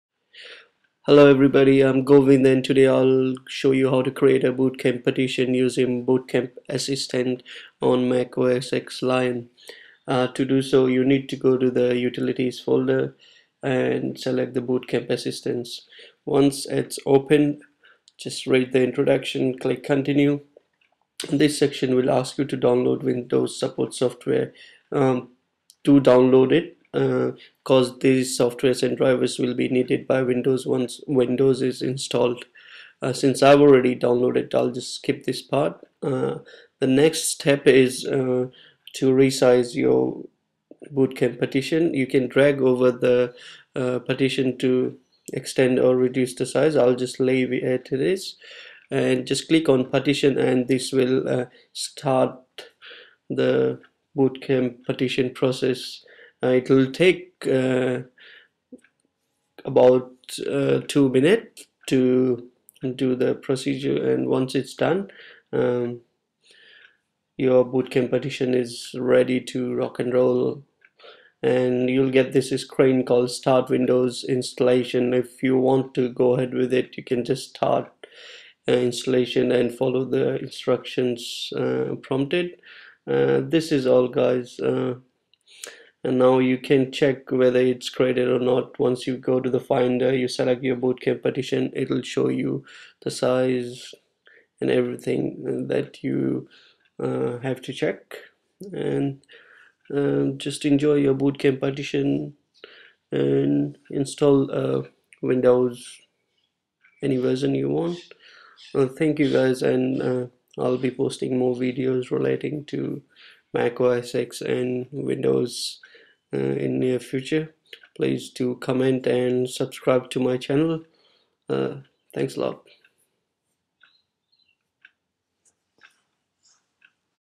How to Create a Bootcamp Partition on Mac .
Instructions :
1) Go to Applications Folder , Open Bootcamp Assistance
2) Click on Continue
3) Choose Download if u have not downloaded windows support software .( Note you will need a FAT formatted disk to download the windows support file ). click continue.
4: Next you can choose the size of your partition , Default size is 20Gb . Continue after you have selected the size . Not: Windows 7 Prof requires 16GB of space for 32 bit and 20 GB for 64 .

5: You are done this will format the desired partition for windows .